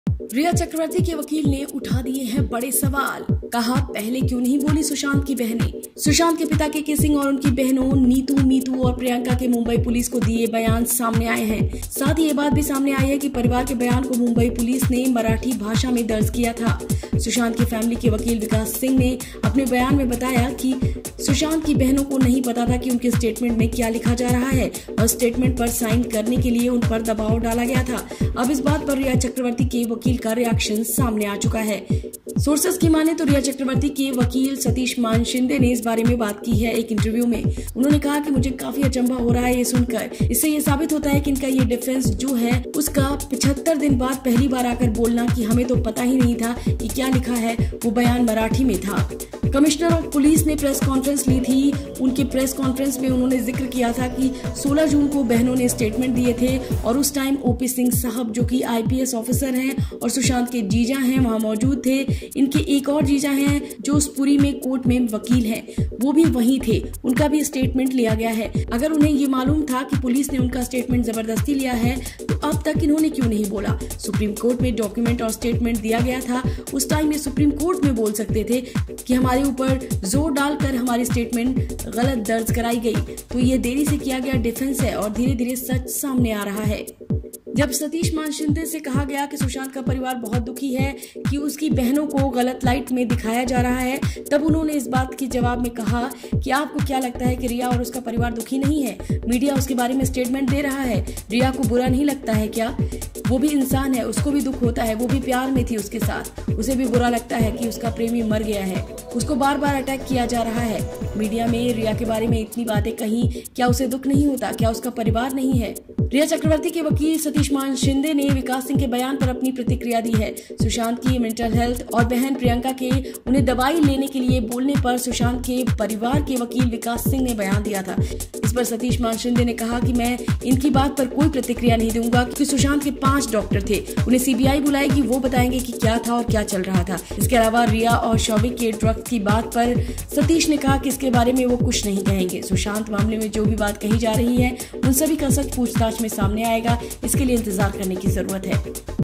Sushant की बहनों पर Rhea Chakraborty के वकील ने उठाए बड़े सवाल | FilmiBeat
Rhea Chakraborty’s lawyer Satish Maneshinde reacts on Sushant's family statement. Watch video to know more.